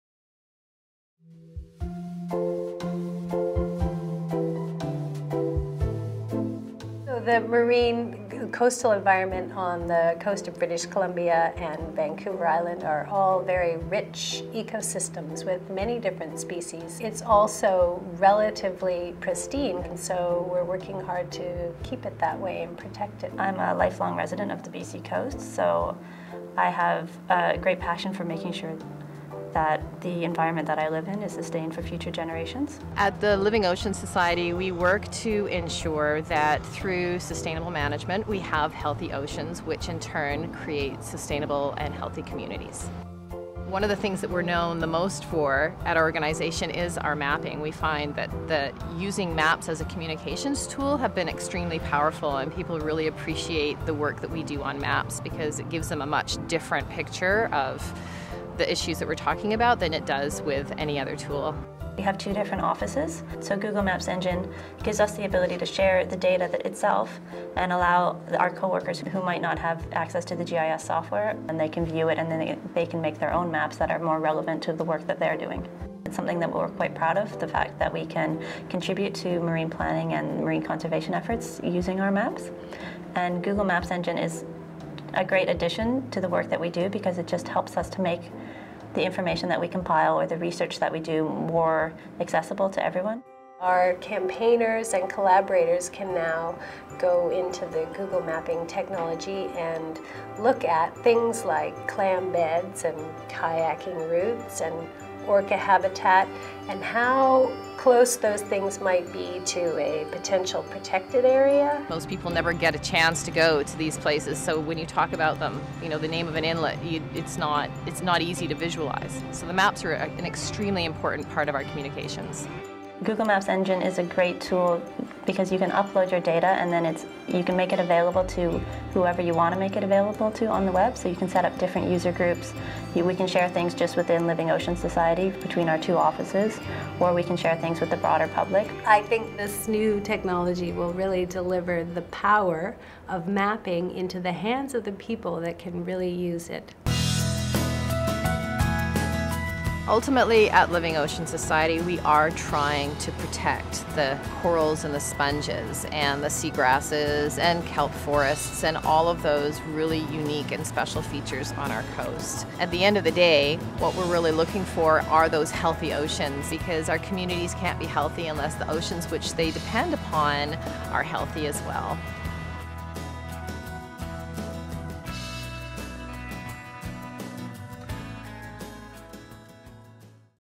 Protecting British Columbia's coastline
Living Oceans Society has been using Google Maps Engine as a way to collaborate on their GIS data internally, as well as publish their maps to a wider audience. Living Oceans Society aims to educate BC residents on why it's important to preserve marine environments.